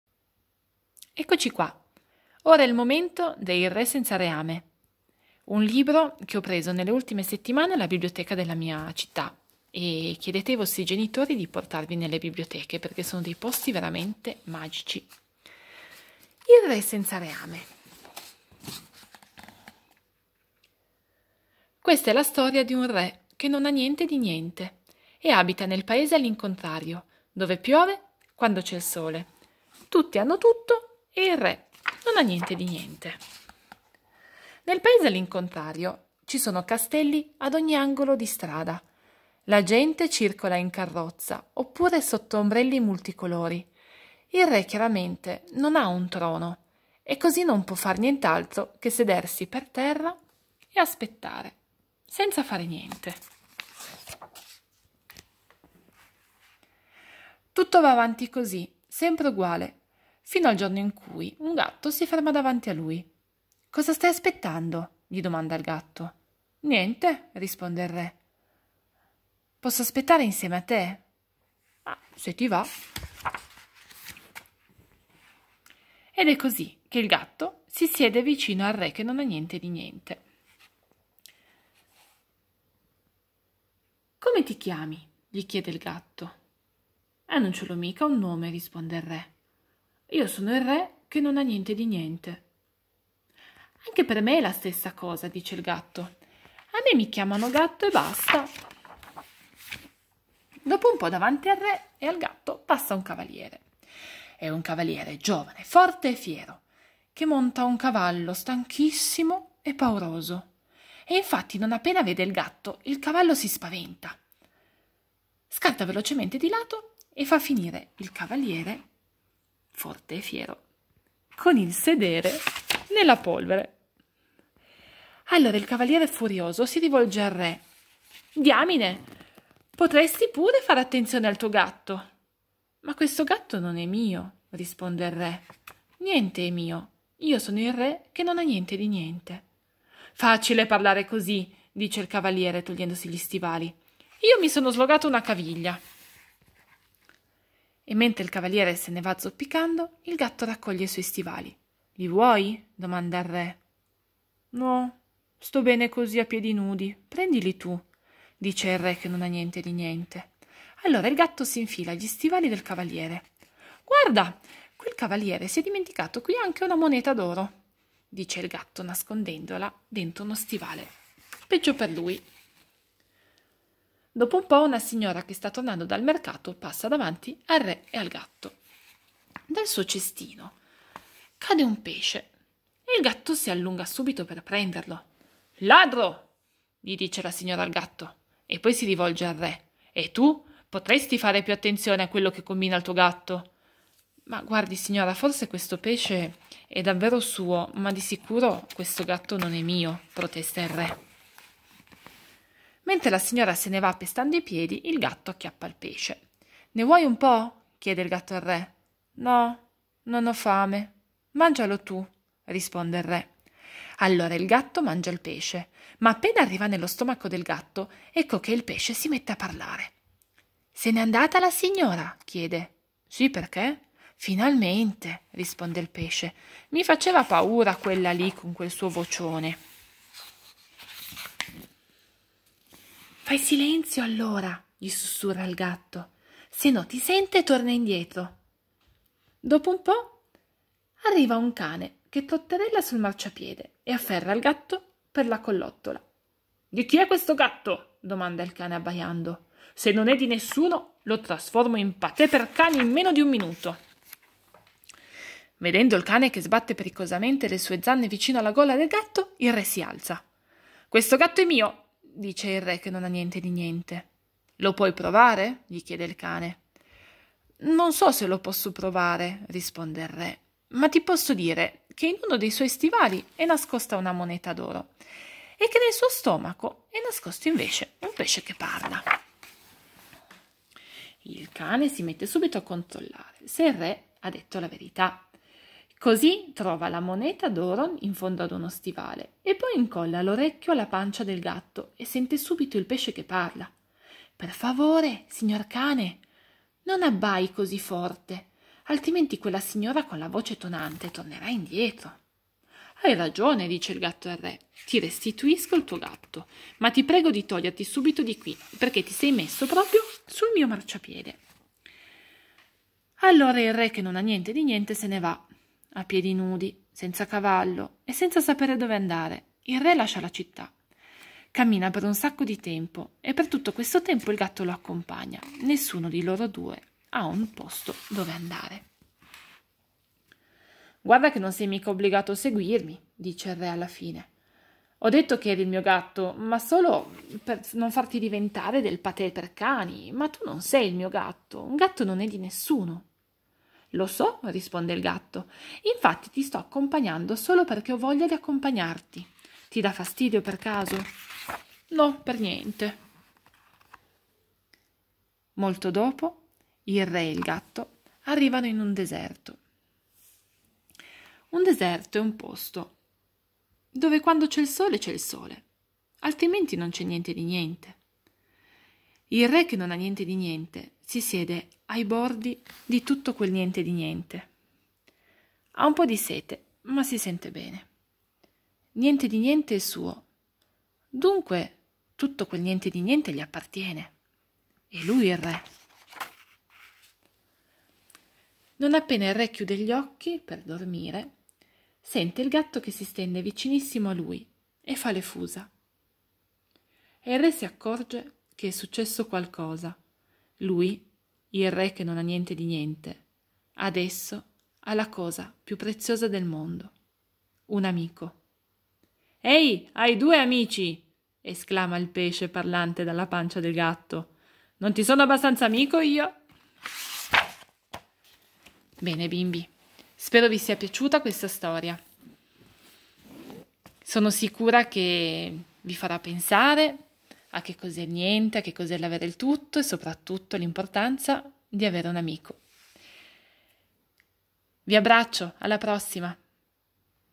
Il re senza reame Lettura per bambini
di Alex Cousseau e Charles Dutertre
L'importanza delle piccole cose, il valore dell'amicizia.